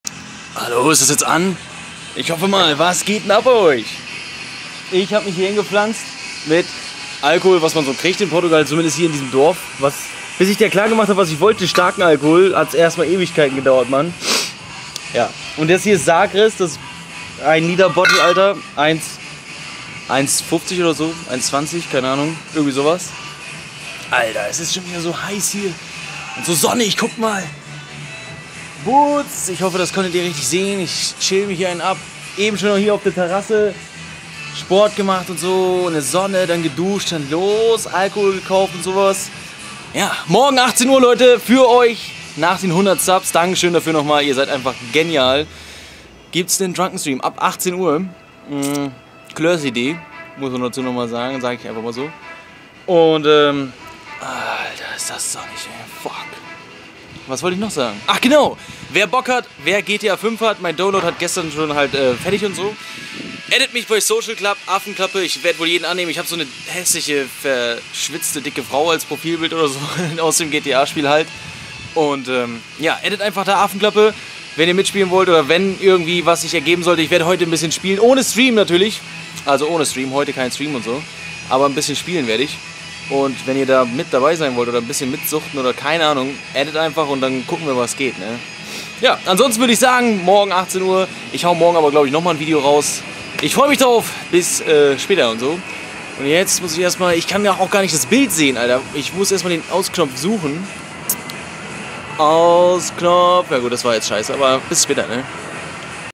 100 Subs! DANKE! Euer DrunkenStream Morgen 18 Uhr!! GTA5??
Einfach hamma euer Support danke! :D

Morgen euer DrunkenStream um 18 Uhr!! :D

UND
NEUE SUBICONS INC!!